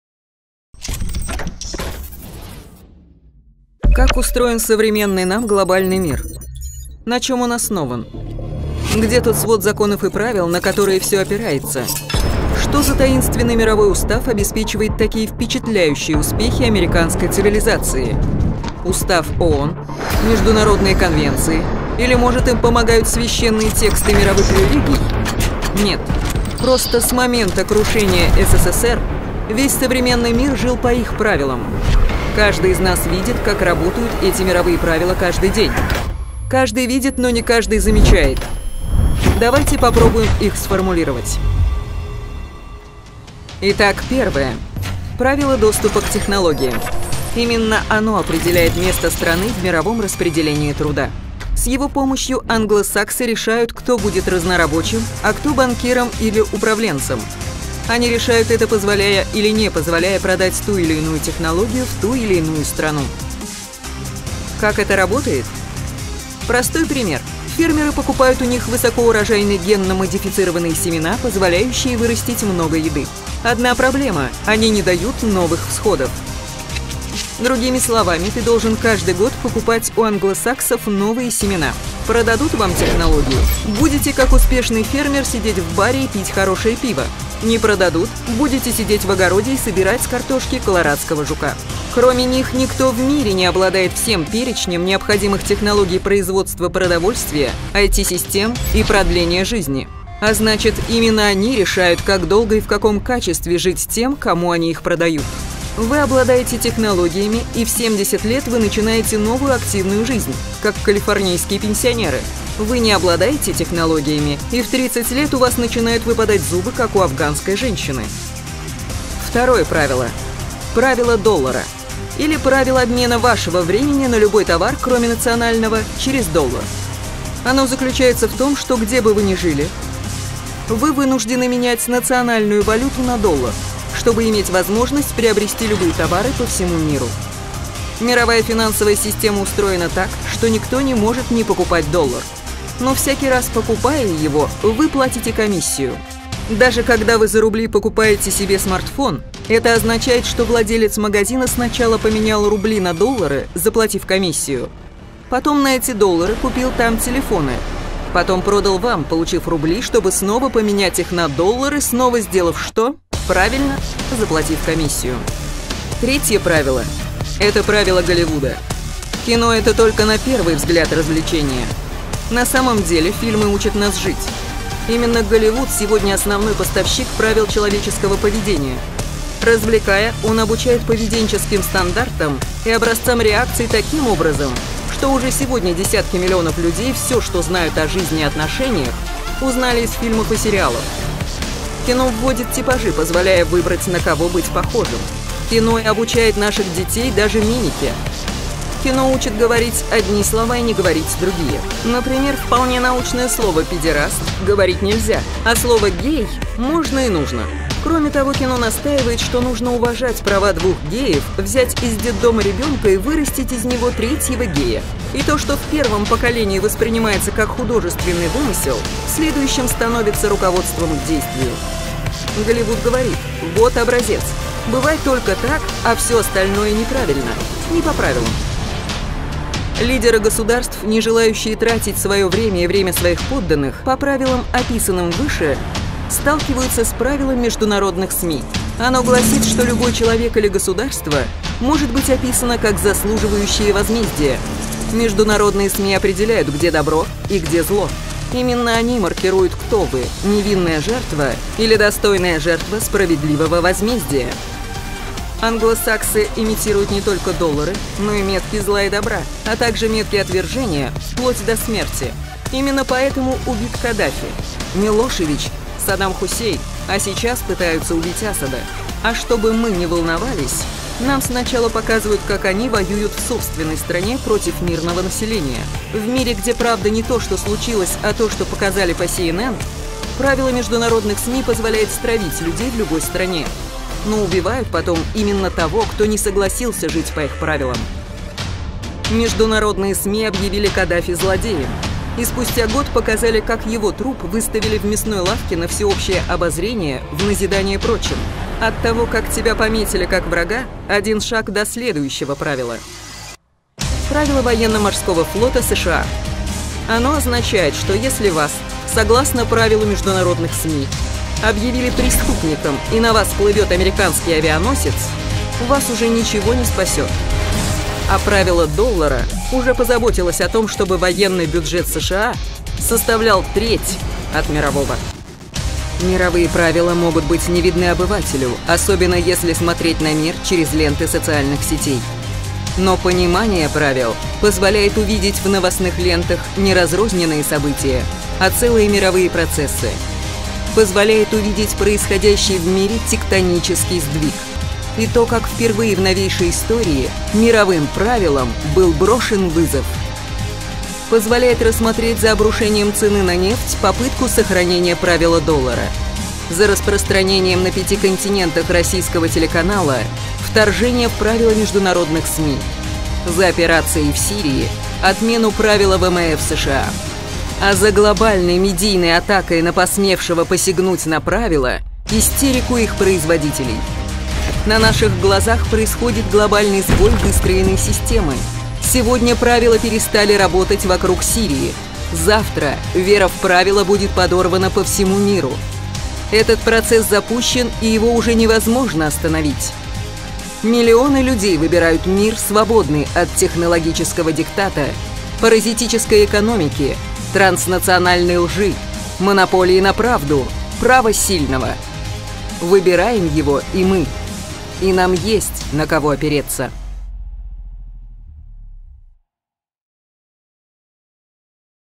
5 światowych reguł
W świecie panują reguły.

Stworzono je i rozpowszechniono za oceanem. Świat żyje według tych reguł. Jeśli ich nie przestrzegasz, to jesteś zagrożeniem, odmieńcem lub ofiarą. A grając zgodnie z nimi – zawsze przegrywasz.

W niniejszym filmie sformułowaliśmy te światowe reguły, ukazaliśmy jak każda z nich działa i zaproponowaliśmy przyczynę tego, że Władimir Putin stał się dla Zachodu takim problemem – on jako pierwszy w najnowszej historii rzucił wyzwanie światowym regułom.